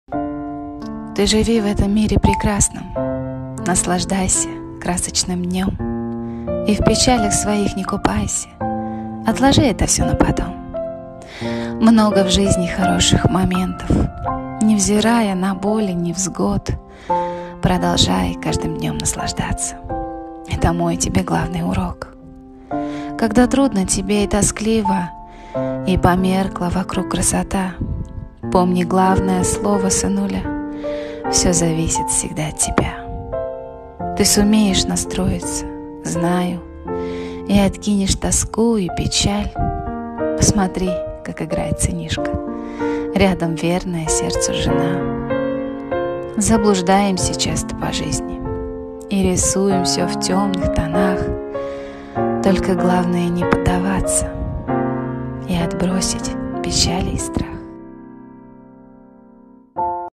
Ты живи в этом мире прекрасном
Наслаждайся красочным днем
И в печалях своих не купайся
Отложи это все на потом.

Много в жизни хороших моментов
Не взирая на боли невзгод
Продолжай каждым днем наслаждаться
Это мой тебе главный урок.

Когда трудно тебе и тоскливо
И померкла вокруг красота
Помни главное слово сынуля
Все зависит всегда от тебя.

Ты сумеешь настроиться - знаю
И откинешь тоску и печаль
Посмотри как играет сынишка
Рядом верная сердцу жена.

Заблуждаемся часто по жизни
И рисуем все в темных тонах
Только главное не поддаваться
И отбросить печали и страх.

Ваше мнение очень важно❗Поддержите нас лайком👍 и комментарием💬.